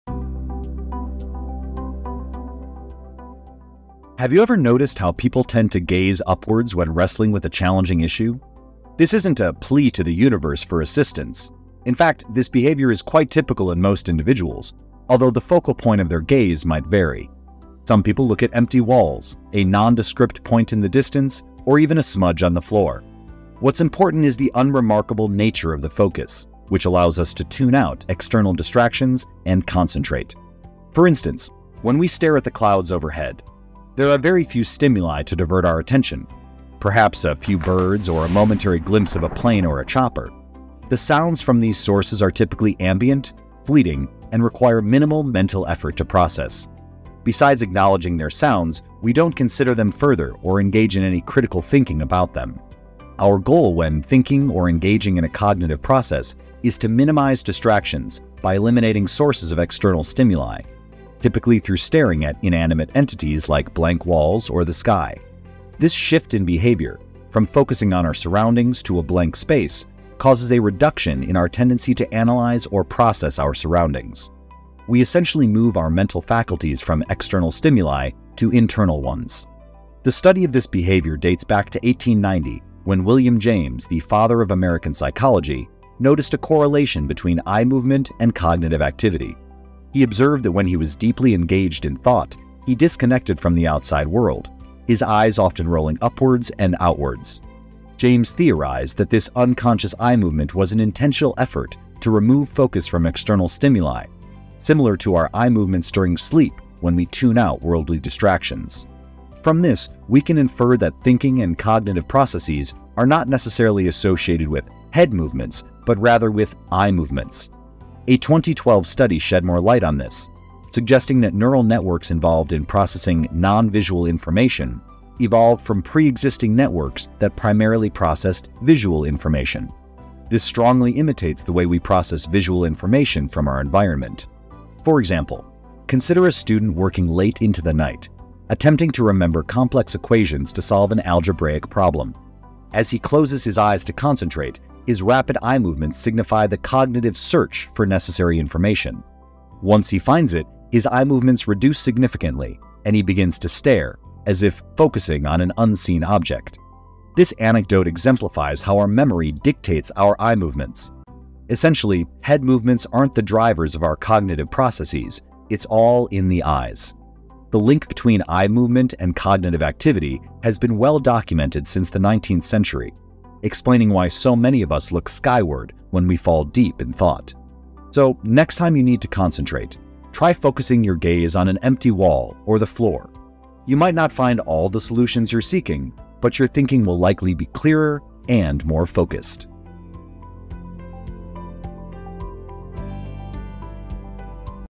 Why Do People Look Up When Thinking?
According to current research, we look up while thinking because the same brain structures involved in thinking evolved from pre-existing neural connections for searching visually. The same reason you move your eyes to search for a physical object may also play a role in your eye’s movements when searching for answers.

Furthermore, it’s easy to see why most of us look to the heavens when thinking, as though we are trying to converse with God. In increasingly desperate times, we might be praying, but we are also actively looking up to tune out the noise of our surroundings.